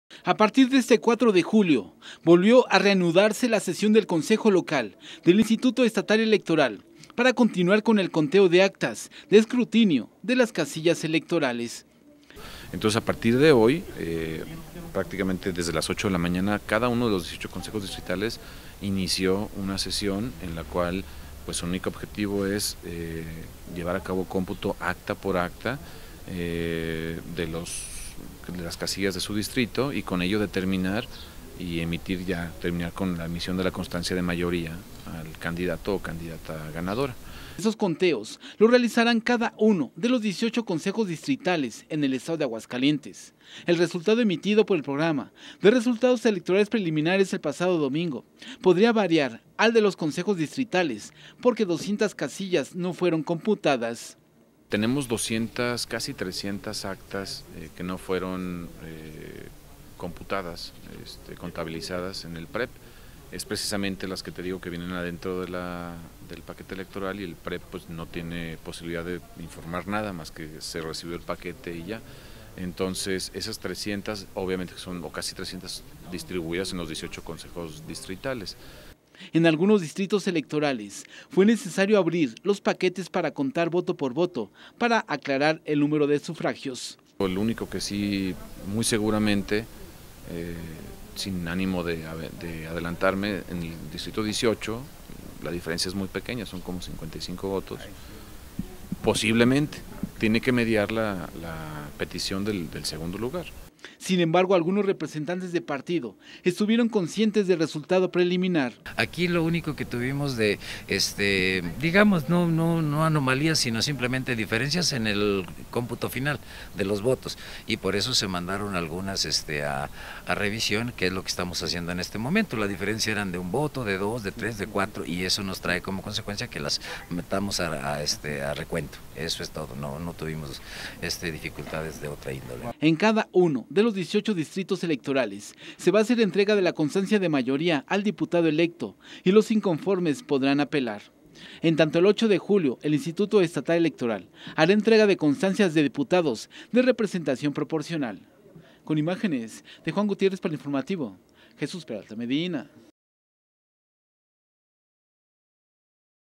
INSTALARON CONSEJO GENERAL
Comenzó el conteo de Consejos Distritales en Aguascalientes.